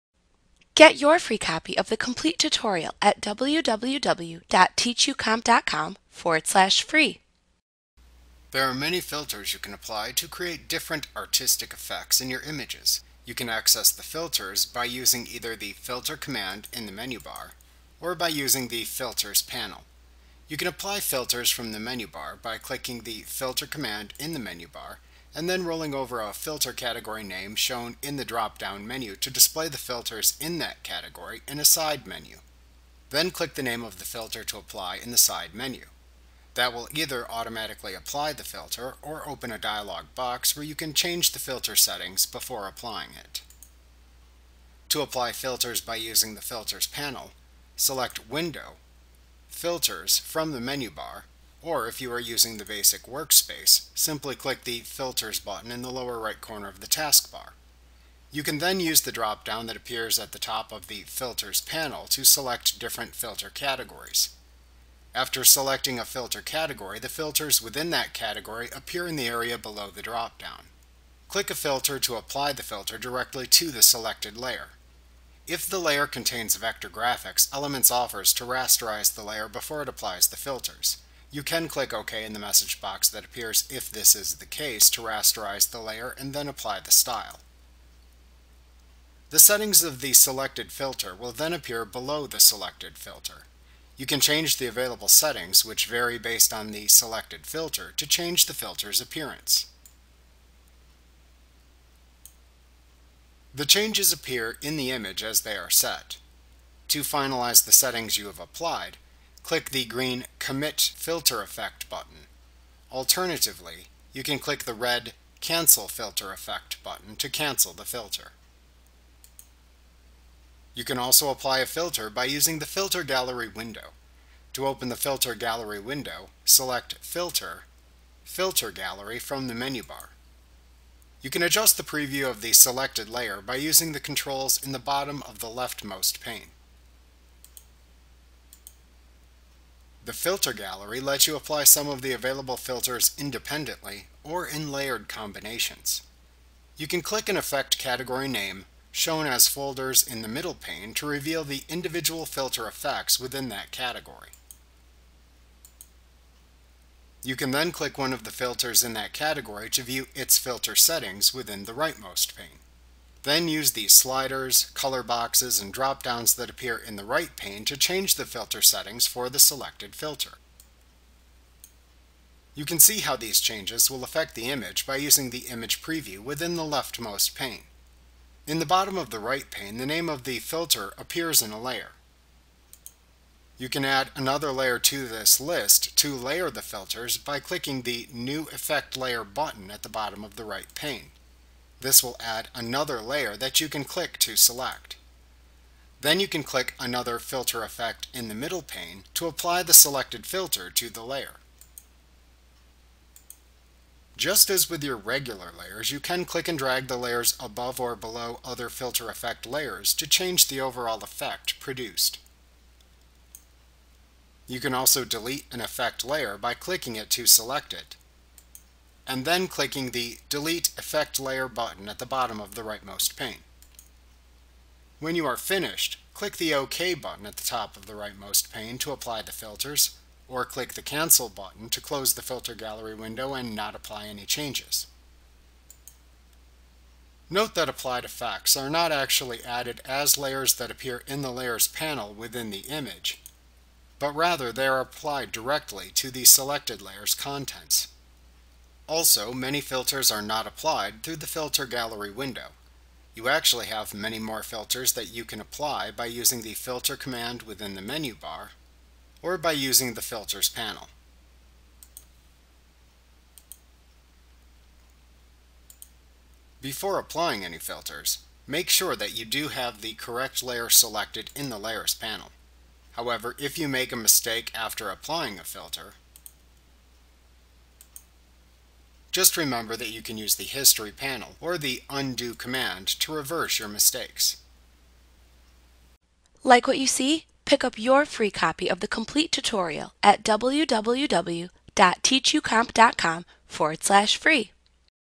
Photoshop Elements 2020 Tutorial Filters & the Filter Gallery Adobe Training
FREE Course! Learn how to use filters and the filter gallery in Adobe Photoshop Elements A clip from Mastering Photoshop Elements Made Easy v. 2020. - the most comprehensive Photoshop Elements tutorial available. Visit us today!